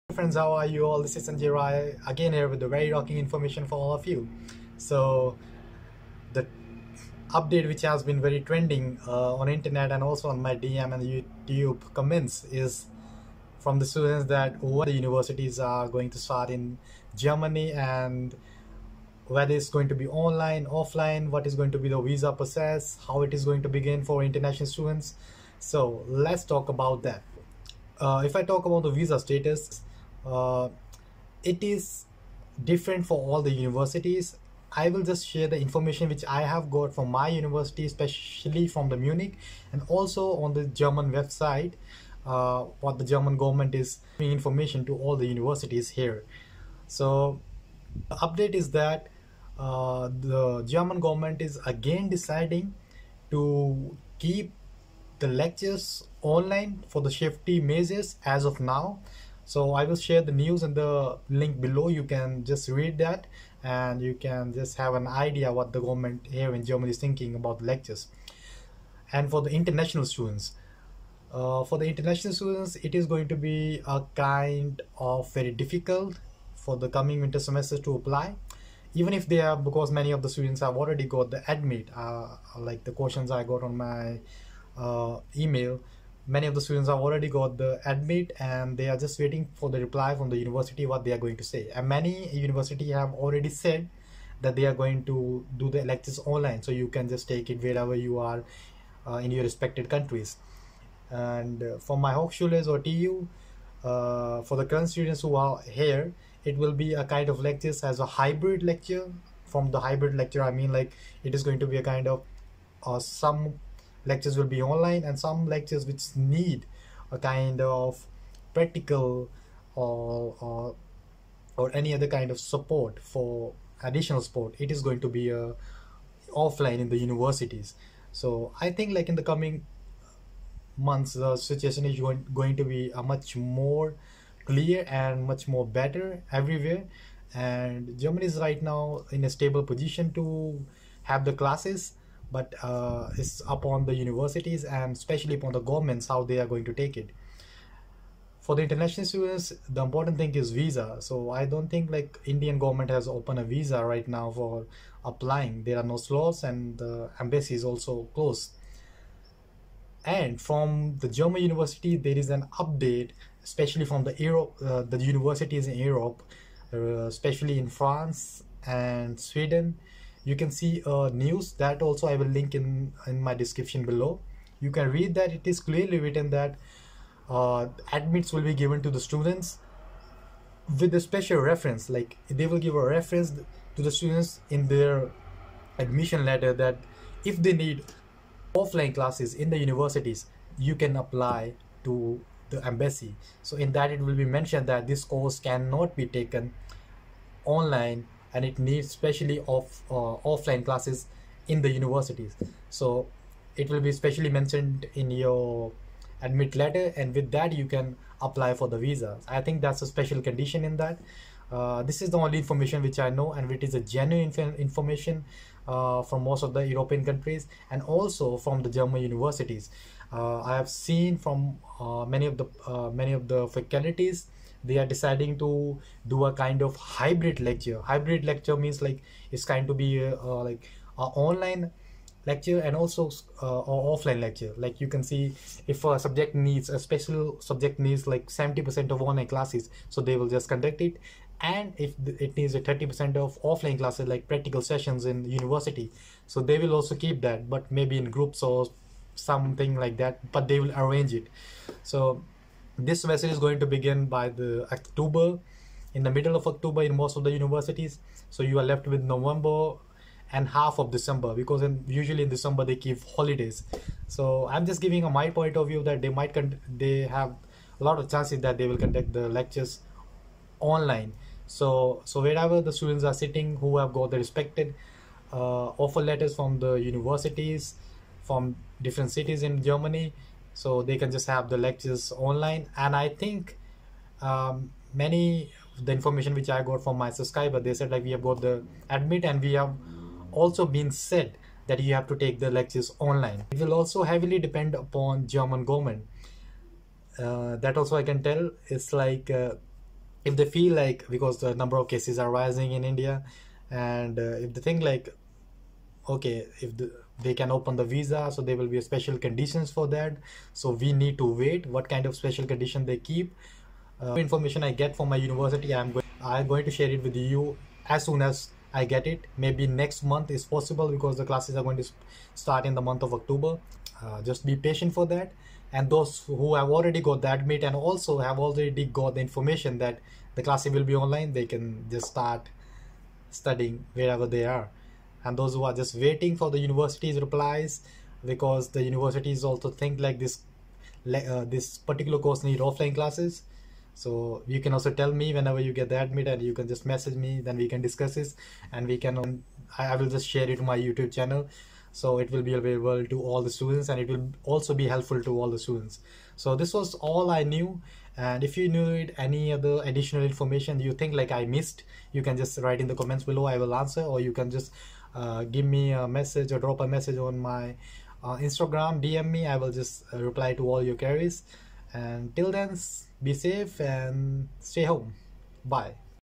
WINTER SEMESTER 2020 UPDATE | VISA INFORMATION FOR STUDENTS
In this video i have discussed all the new of german government how they are planning upcoming semester for international students.
see the full video for all information.
germany new link for students